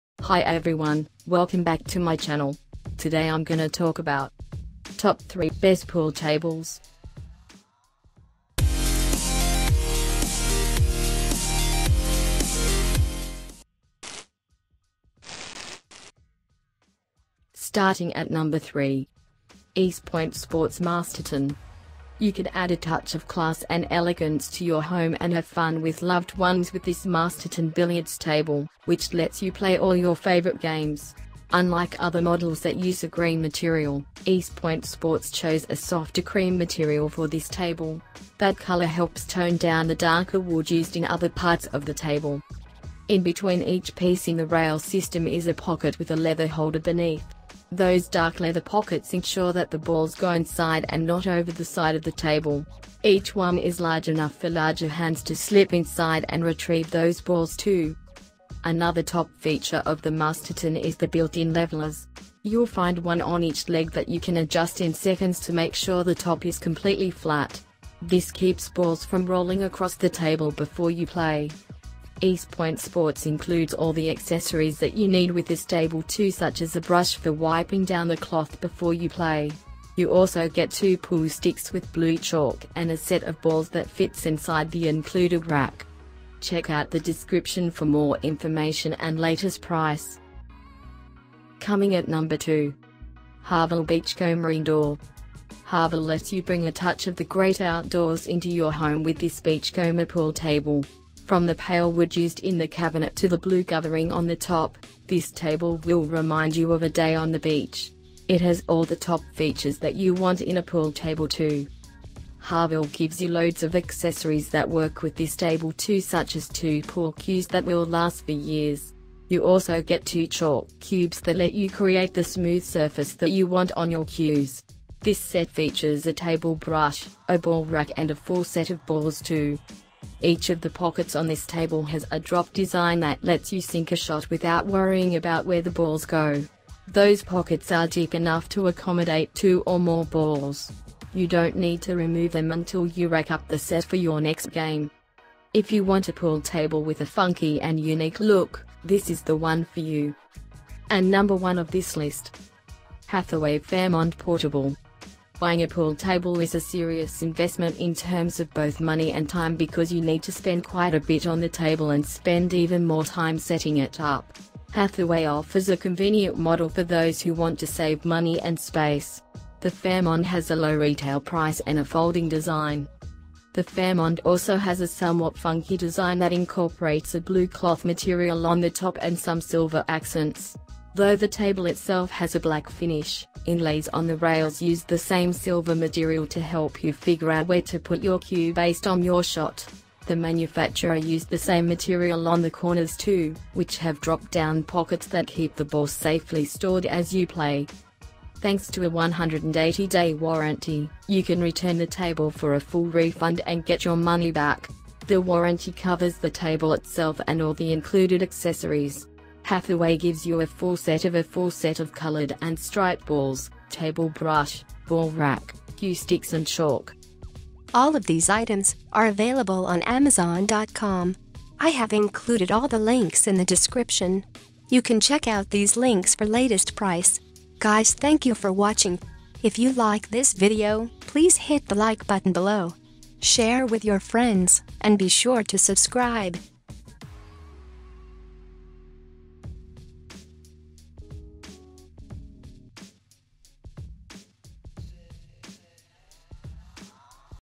Best Pool Tables In 2023? Top 3 Best Pool Tables Reviews
Here is the Updated Prices of the Best Pool Tables in 2023!

Things that we mentioned in this video:
1. Hathaway Fairmont Portable - B015AHRE9C
2. Harvil Beachcomber Indoor - B01M8QIK1T
3. EastPoint Sports Masterton - B071WF6N9F